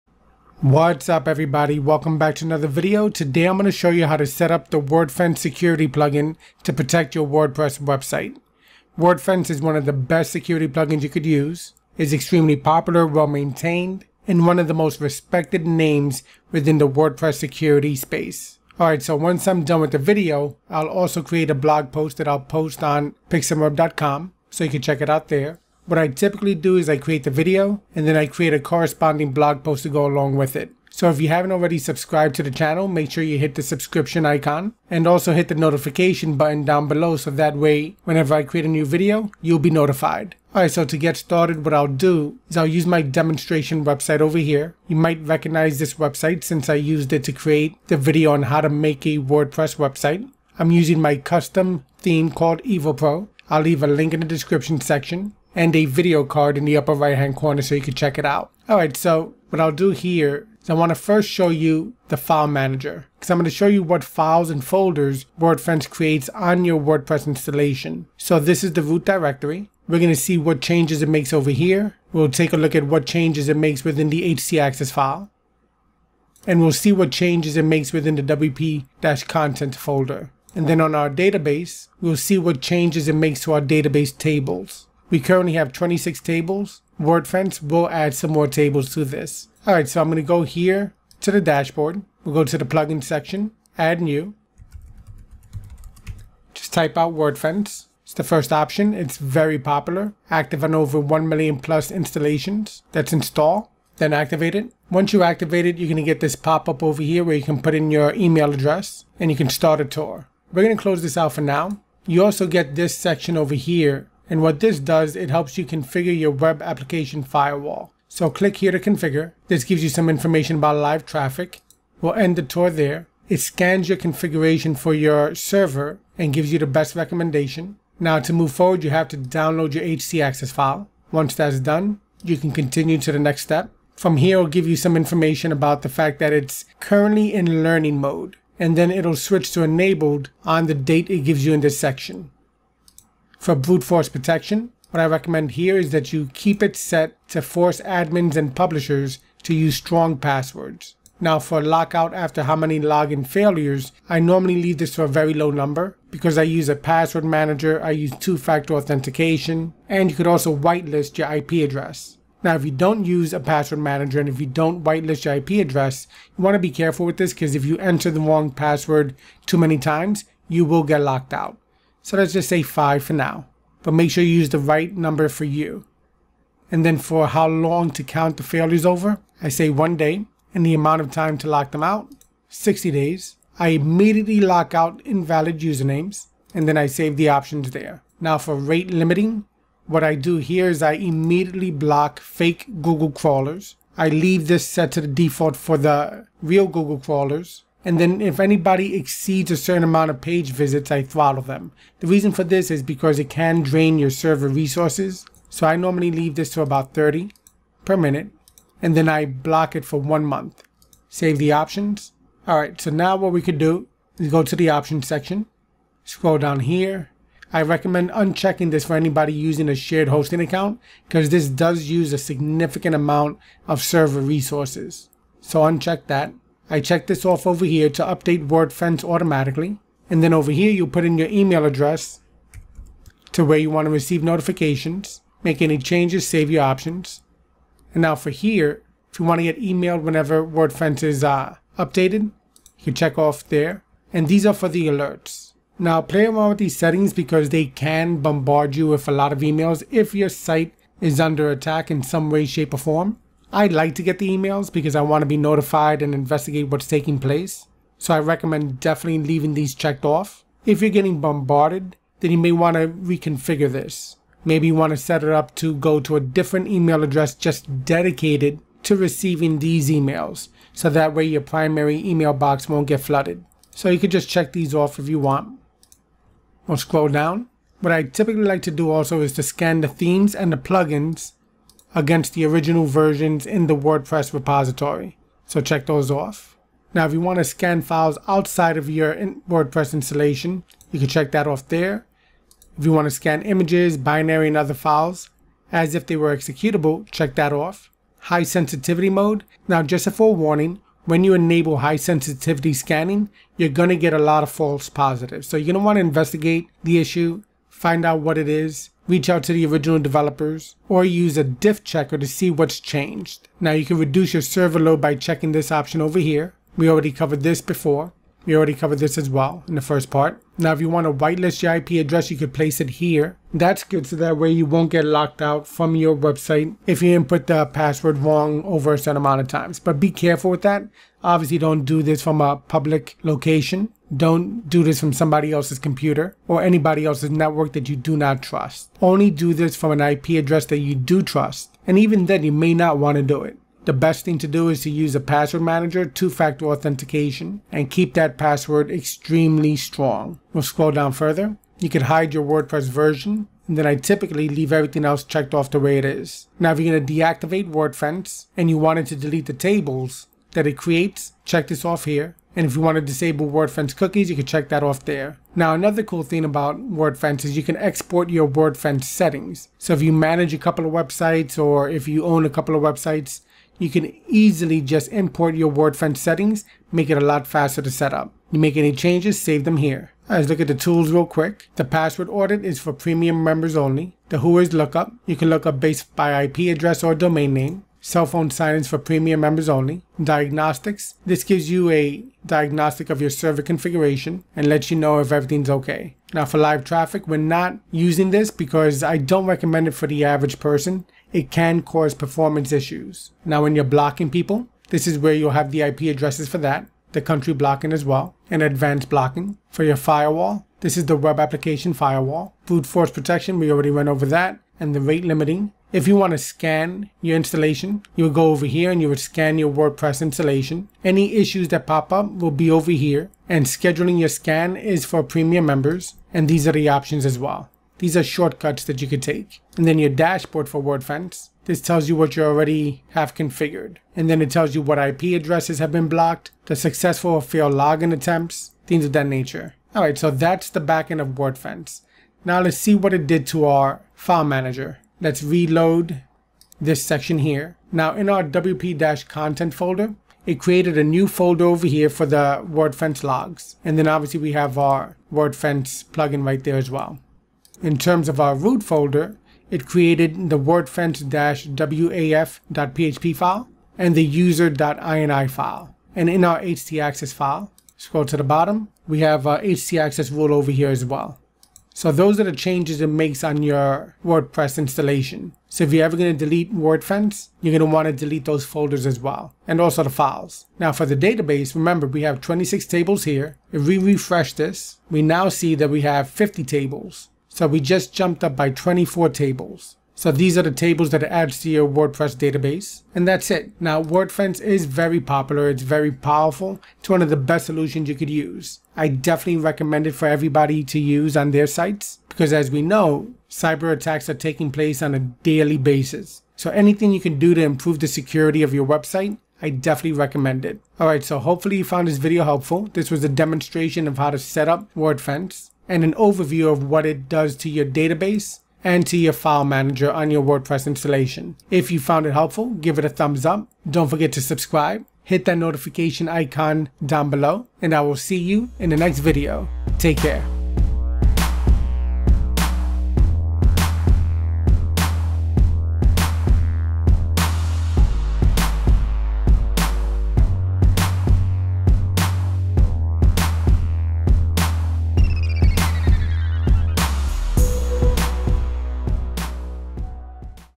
WordPress Security Tutorial - How To Secure WordPress - Wordfence Security Plugin
In this WordPress Security Tutorial Video I will show you how to Secure your WordPress Website using the Wordfence Security Plugin which is one of the best WordPress Plugins to use when securing your website.

I give you an overview of the most important settings for Wordfence and also show you what files and folders it adds to your WordPress Installation.

I then show what database tables Wordfence creates when you install and activate it.

Keeping your website secure is extremely important and Wordfence does a great job at preventing the vast majority of attacks.